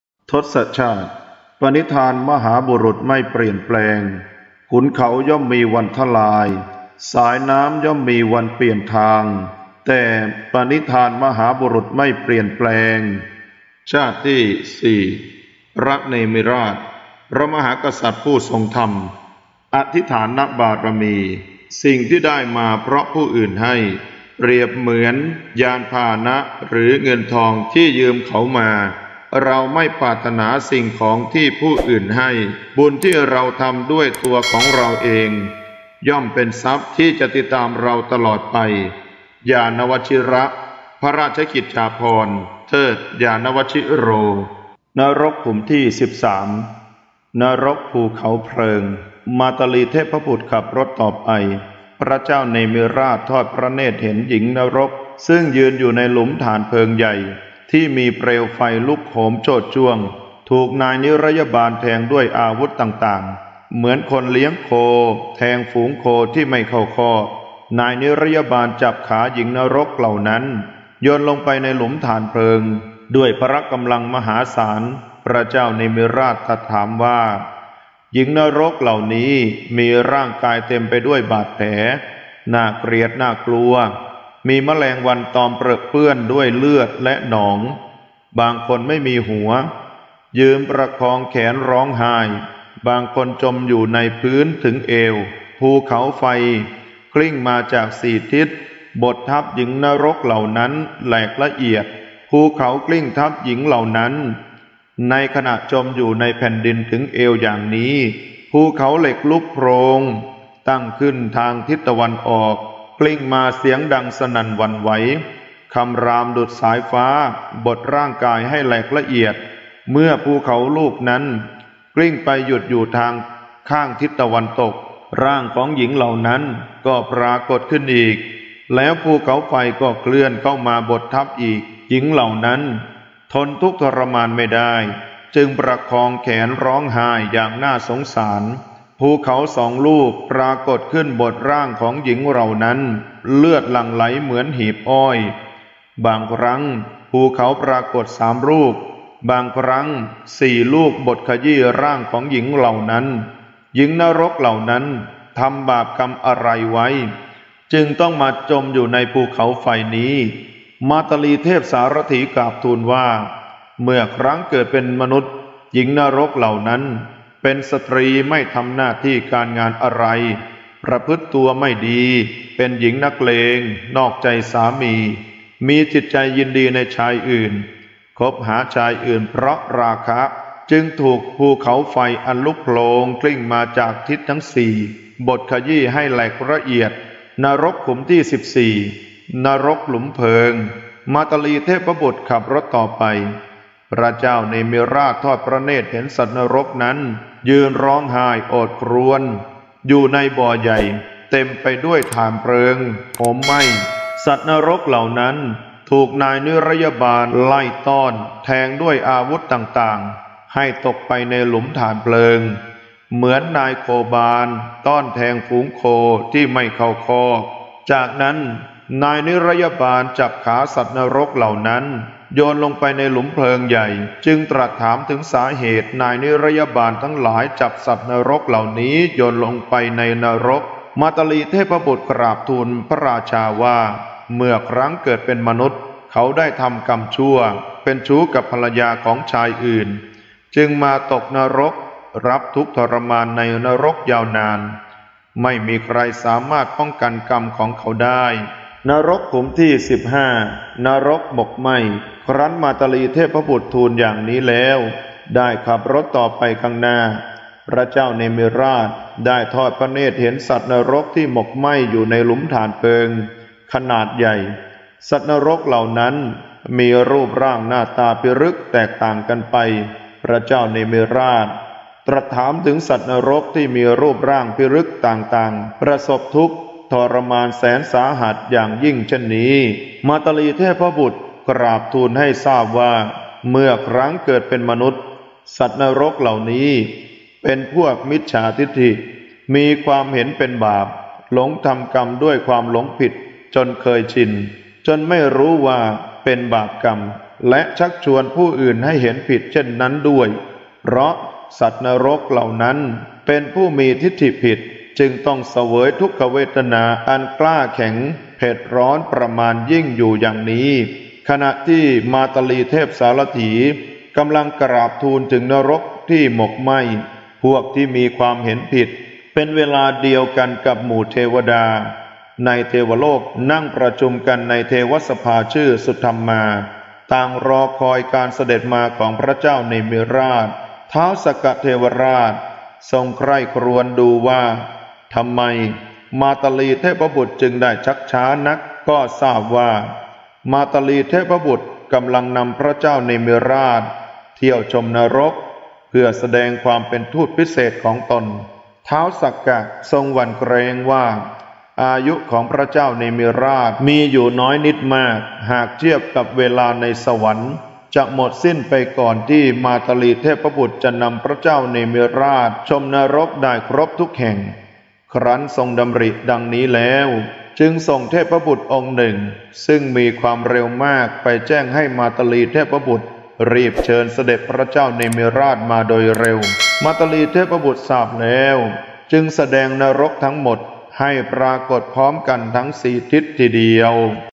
ทศชาติ 032 พระเนมิราช นรกขุมที่ 13 นรกภูเขาเพลิง
ทศชาติเป็นเรื่องราวในอดีตชาติของพระพุทธเจ้า ขณะที่พระองค์กำลังบำเพ็ญบารมีเพื่อบรรลุพระโพธิญาณ และเป็นเรื่องราวทางพระพุทธศาสนาที่ชาวโลก ไม่เฉพาะแต่คนไทย รู้จักมาที่สุดเรื่องหนี่ง บรรพบุรุษไทยในอดีต ได้นำทศชาติมาเขียนเป็นภาพจิตรกรรมฝาผนังพระอุโบสถตามวัดต่างๆ บอกเล่าเรื่องราวให้ลูกหลานได้ศึกษาเส้นทางชีวิตมหาบุรุษ ผู้ที่จะเกิดมาเป็นพระศาสดาเอกของโลก